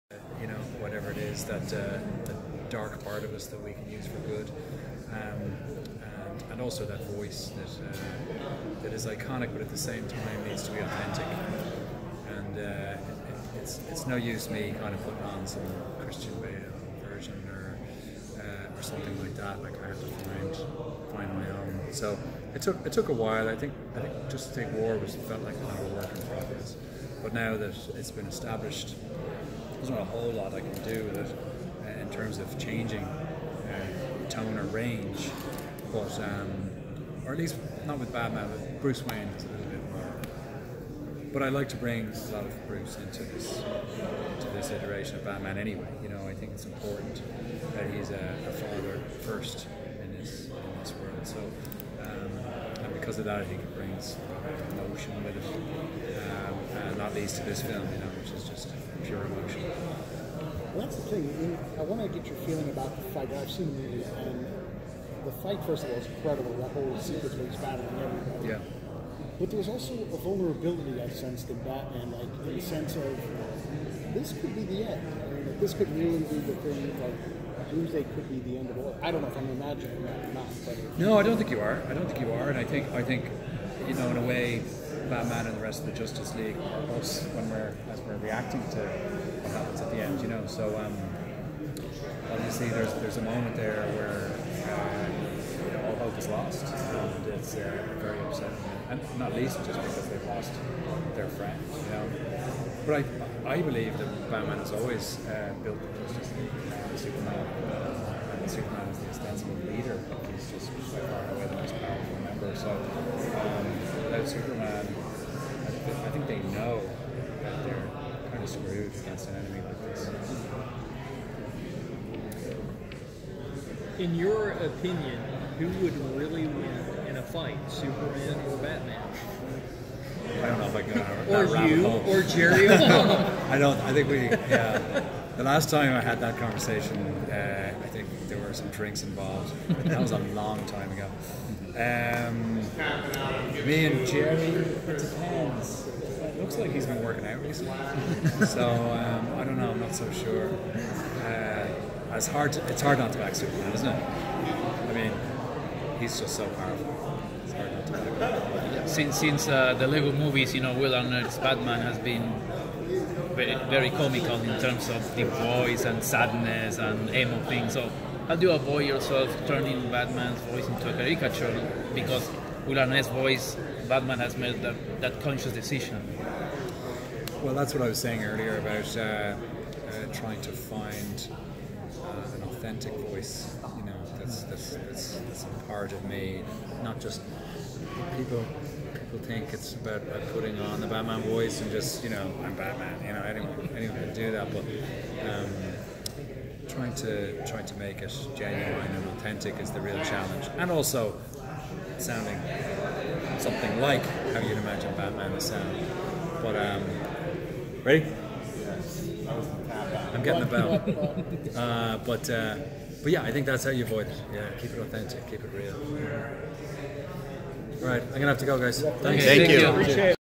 Jason O'Mara talks "The Death of Superman " at SDCC 2018
Jason (Batman) talks Batman at "The Death of Superman " Presser